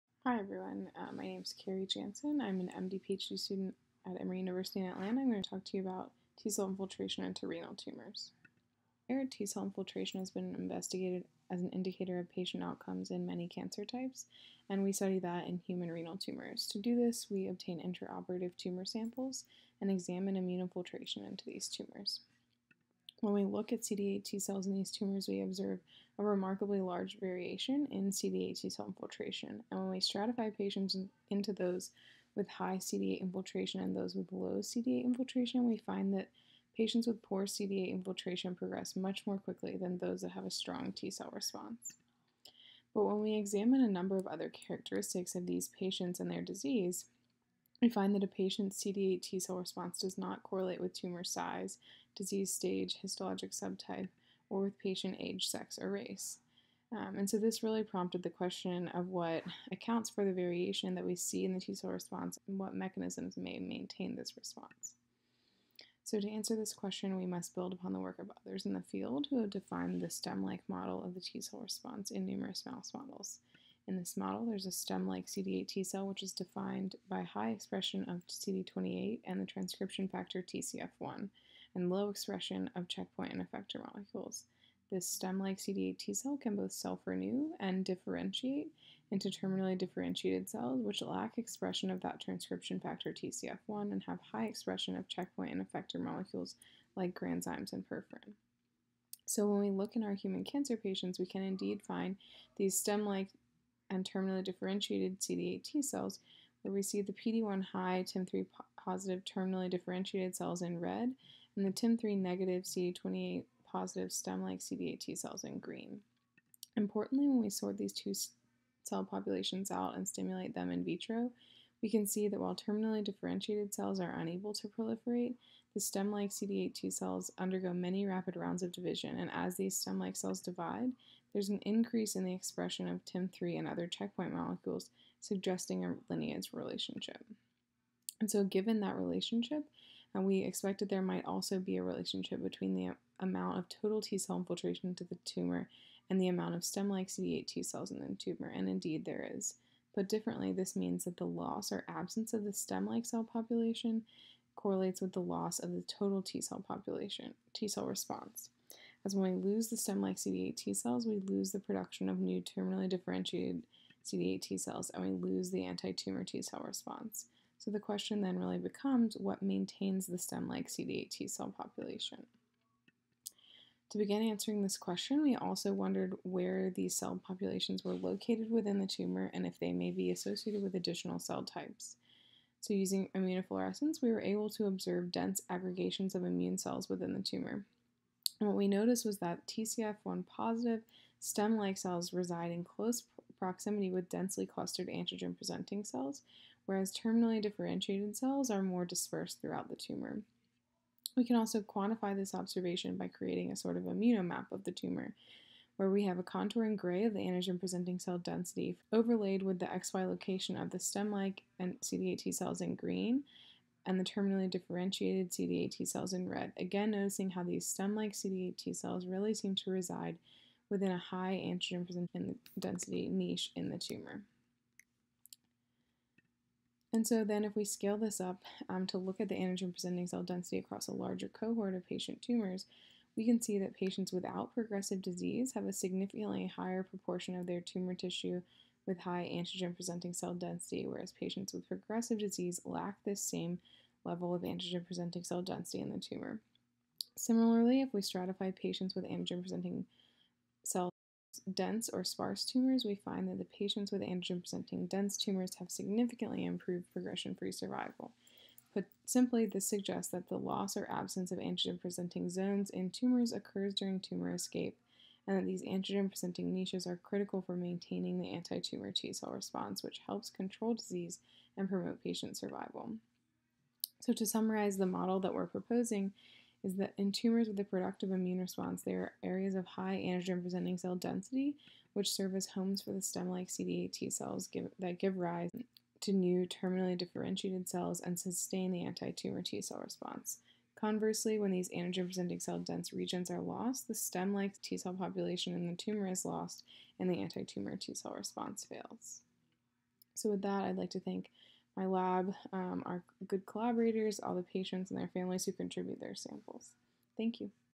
2020 APSA Virtual Meeting Presentation: Carey Jansen
"CD8 T-Cell Infiltration into Renal Tumors Requires a Stem-Like Cell & an Antigen Presenting Niche" is presented by Carey Jansen, an MD/PhD student at Emory University School of Medicine. This presentation was submitted as part of the 2020 American Physician Scientists Association Virtual Meeting.